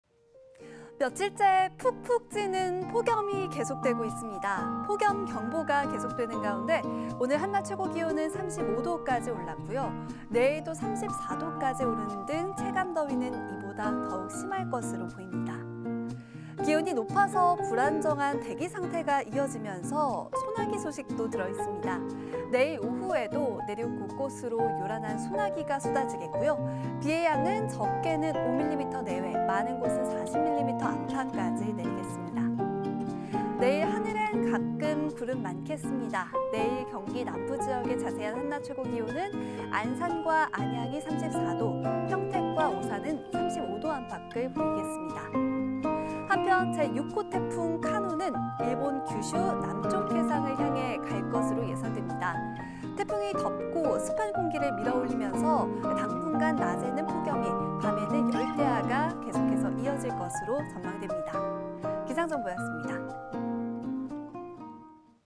08월 02일 기상 정보
며칠째 푹푹 찌는 폭염이 계속되고 있습니다.
폭염경보가 계속되는 가운데
오늘 낮기온은 35도까지 올랐고
내일도 34도 안팎을 보이겠는데요.
체감 더위는 이보다 더욱 심할 것으로 보입니다.

기온이 높아서 불안정한 대기 상태가 이어가 이어지면서
소나기 소식도 들어있습니다.
내일 오후에도 내륙 곳곳으로 요란한 소나기가 쏟아지겠고요.
비의 양은 적게는 5mm 내외,
많은 곳은 40mm 안팎까지 내리겠습니다.

내일 하늘에 가끔 구름 많겠습니다.
자세한 한낮 최고기온은
안산과 안양 34도 오산과 평택 35도가 예상됩니다.

한편 제6호 태풍 카눈은
일본 규슈 남쪽 해상을 향해 갈것으로 예상됩니다.
태풍이 덥고 습한공기를 밀어올리면서
당분간 낮에는 폭염이 밤에는 열대야가 기승을 부리겠습니다.